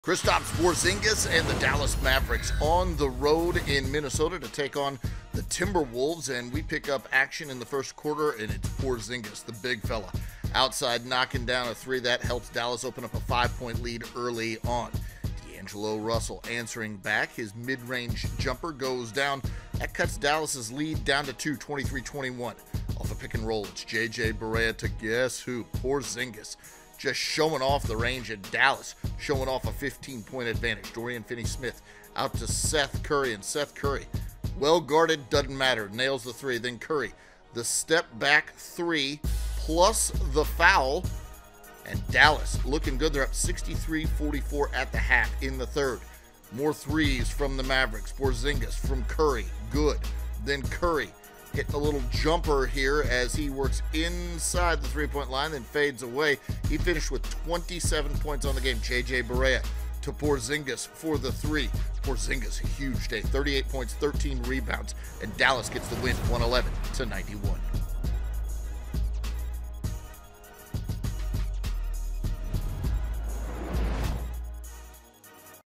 Minnesota Timberwolves vs Dallas Mavericks | March 1, 2020
The Mavericks defeated the Timberwolves, 111-91. Kristaps Porzingis led the way for the Mavericks with a season-high tying 38 points (career-high tying 6 3pt FG), along with 13 rebounds and 5 blocks, while D’Angelo Russell tallied 16 points, 9 rebounds and 7 assists for the Timberwolves in the losing effort. The Mavericks improve to 37-24 on the season, while the Timberwolves fall to 17-42.